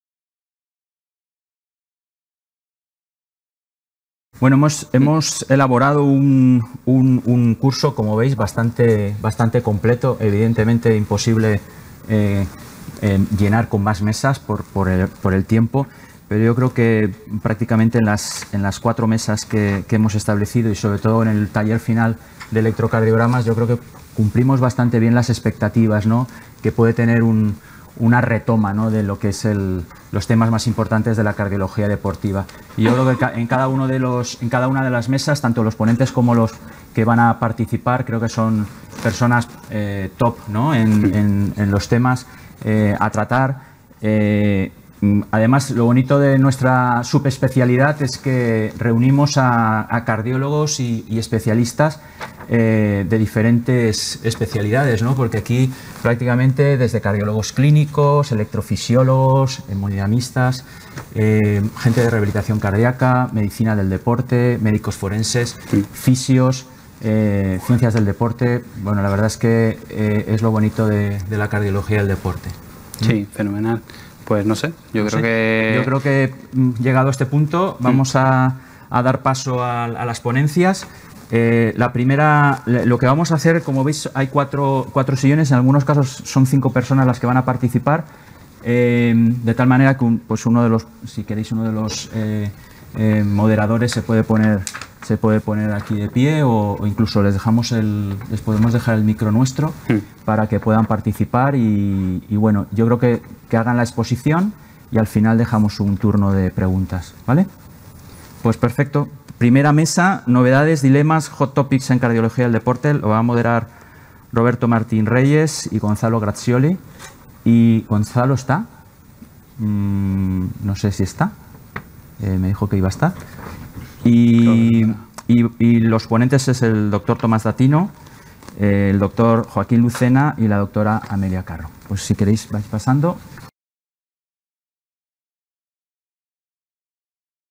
Presentación del Curso Monográfico de Cardiología del Deporte
Los Dres. Fernando de la Guía Galipienso y Leonel Díaz González presentan el V Curso Monográfico Cardiología del Deporte, celebrado en la Casa del Corazón el 30 de junio de 2023.